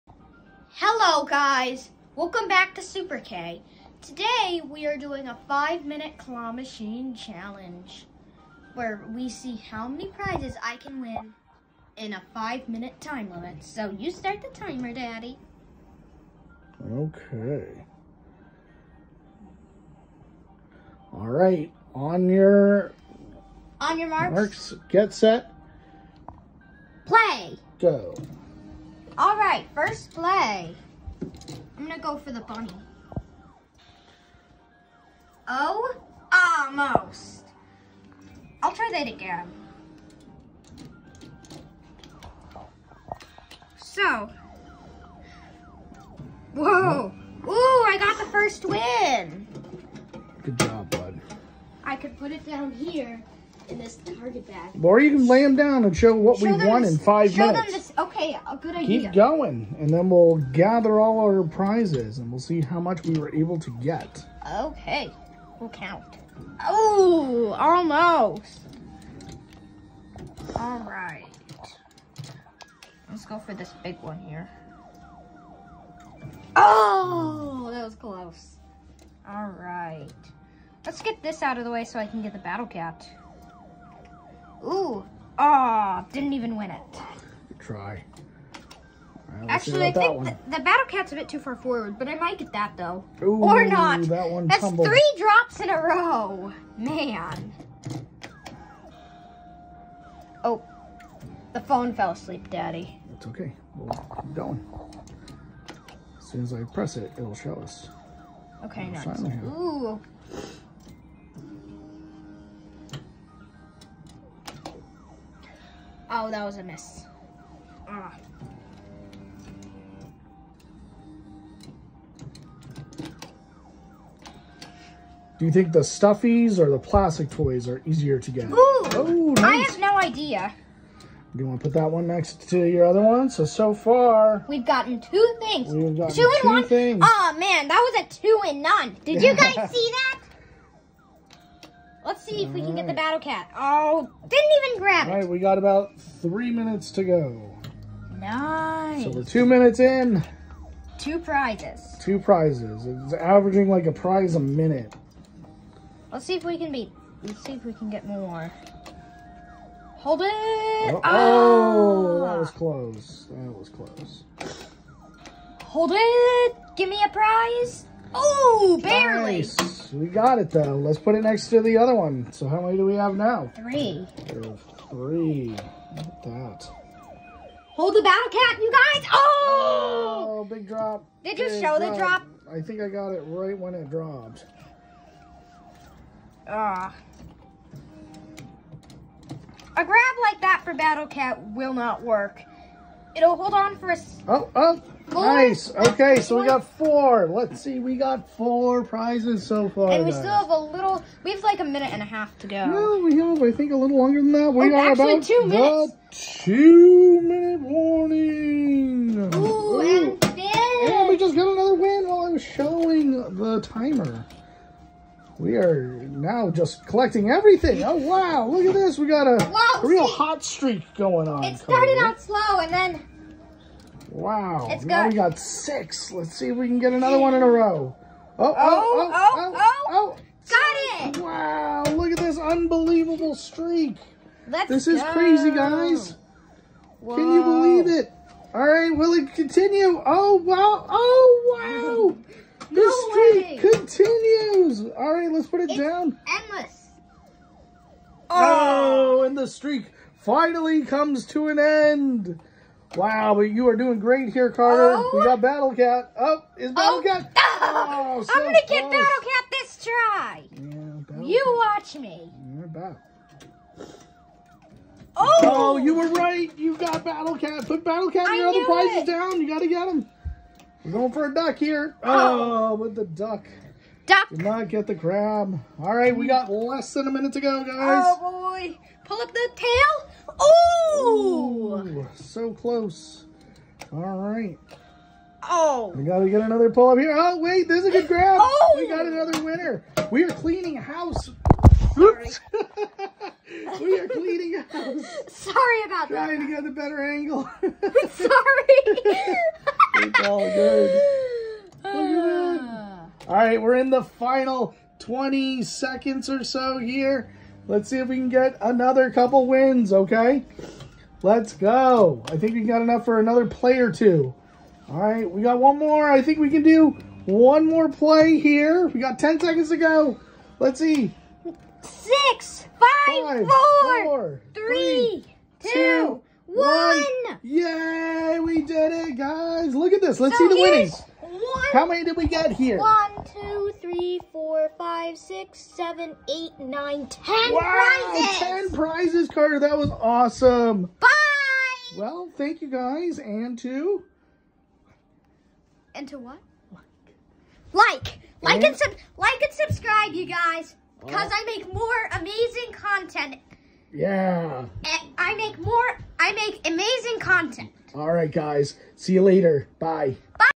Claw Machine 5 Minute Challenge
How many toys can I win from my home claw machine in five minutes?